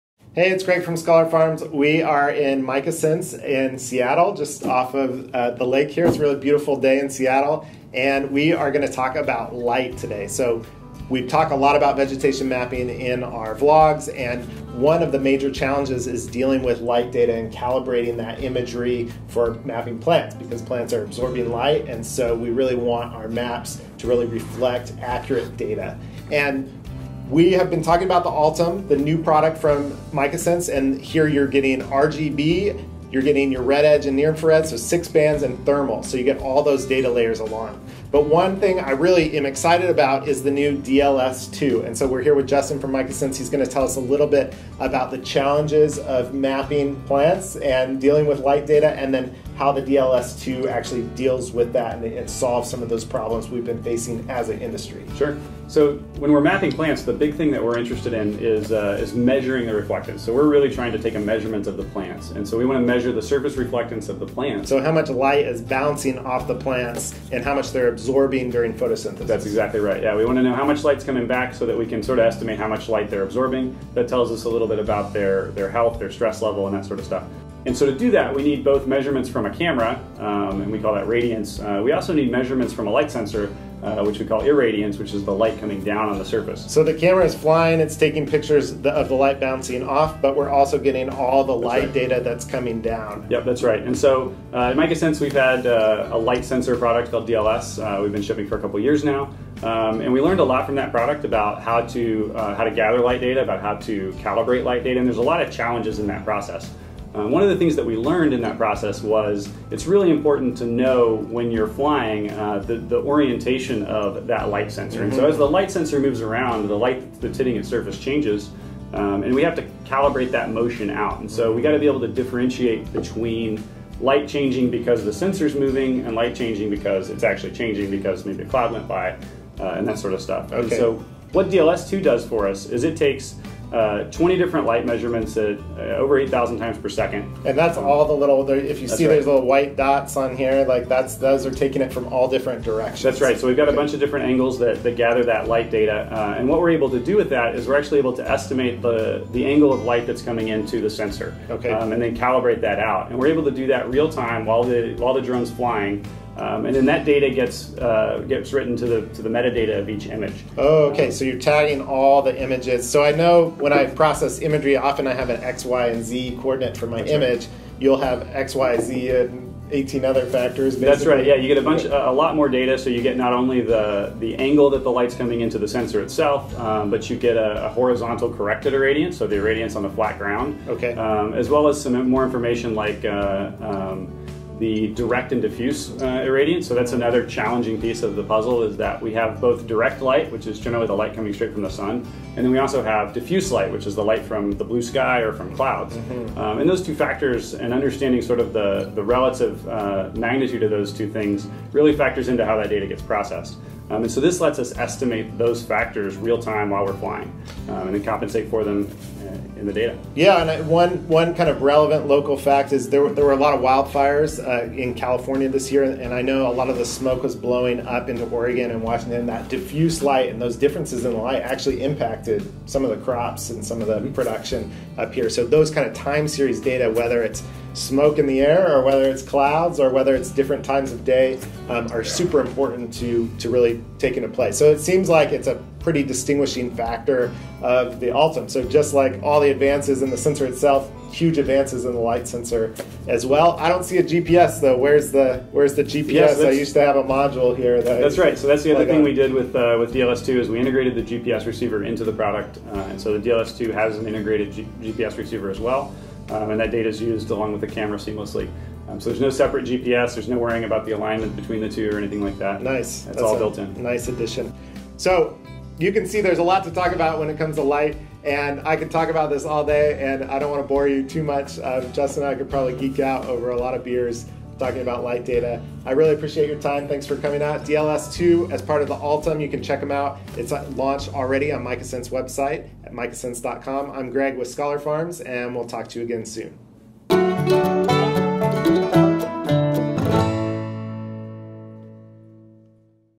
Big strides in light data capture for the new Micasense Altum
Greg Crutsinger from Scholar Farms sits down with Justin from Micasense to discuss the major challenges is dealing with light data and calibrating imagery when mapping plants with drones. The Altum sensor from Micasense  collects 6 bands, RGB, Red edge,  near infrared, along with a low resolution thermal. So all those data layers are aligned.

Micasense has had a light sensor product called the DLS for a couple years now and  learned a lot from that product about how to how to gather light data, about how to calibrate light data. There's a lot of challenges in that process. One of the things that they learned in that process was it's really important to know when you're flying, the orientation of that light sensor and so as the light sensor moves around the light thats hitting its surface changes, we have to calibrate that motion out.

It is important  to differentiate between light changing because of the sensors moving and light changing because it's actually changing because maybe a cloud went by. So what DLS2 does is it takes 20 different light measurements at over 8 thousand times per second. There are many different angles that gather that light data to better estimate the angle of light that's coming into the sensor